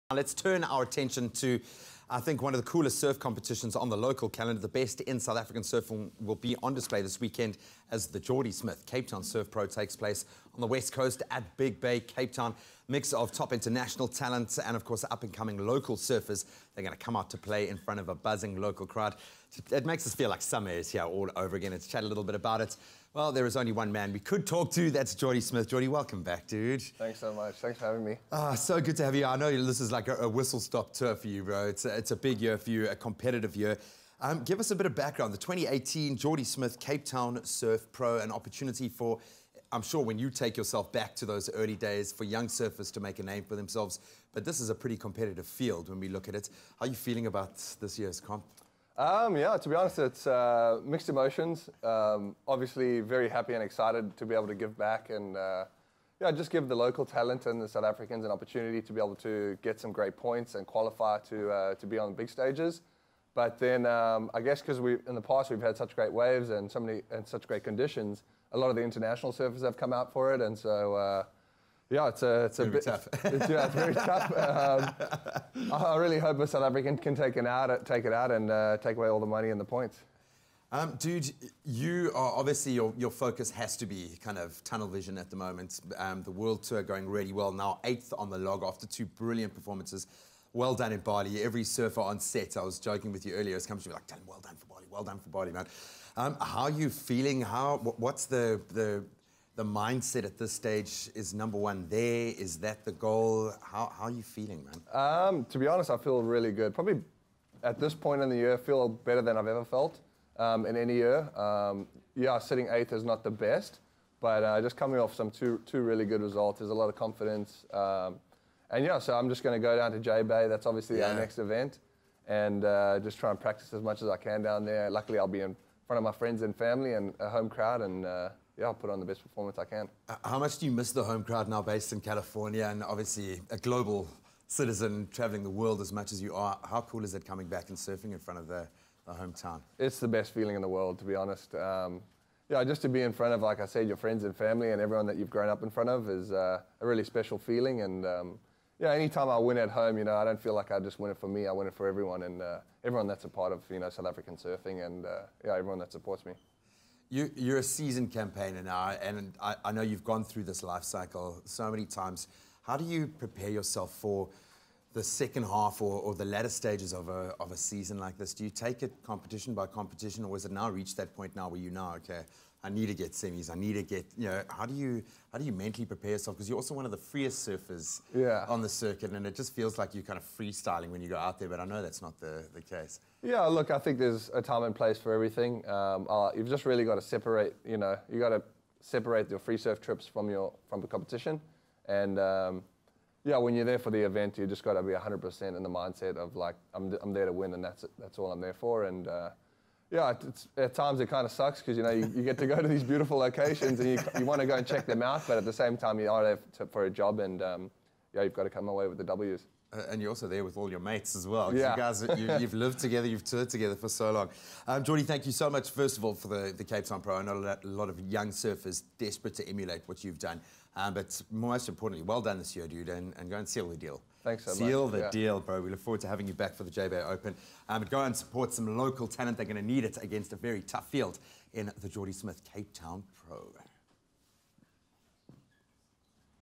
Jordy Smith previews the 2018 Jordy Smith Cape Town Surf Pro
The Jordy Smith Cape Town Surf Pro takes place in Big Bay, Cpt this weekend. To chat about the Pro, we are joined this morning by South African surfing icon Jordy Smith.